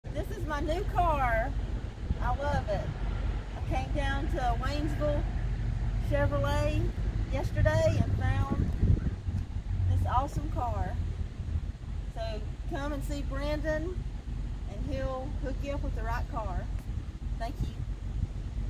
AutoStar Waynesville Customer Testimonial: Michele
Congrats to Michele on her brand new 2019 Chevrolet Spark!

Welcome to the AutoStar Family Michele.